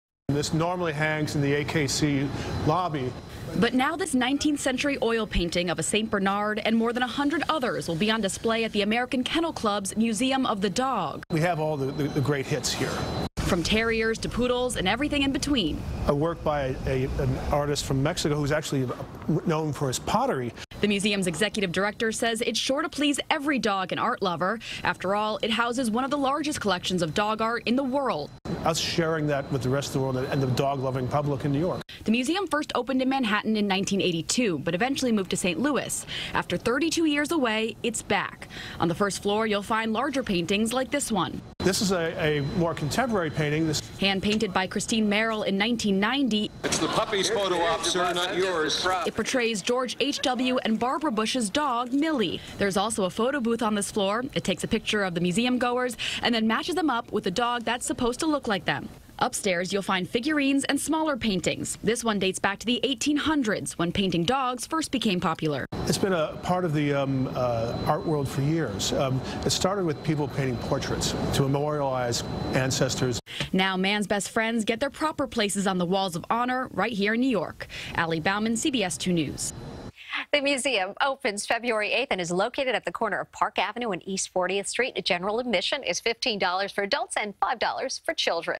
Museum Of The Dog Celebrates Canines Through The Centuries
American Kennel Club is returning its collection of painting around man's best friend back to Manhattan. CBS2's Ali Bauman reports.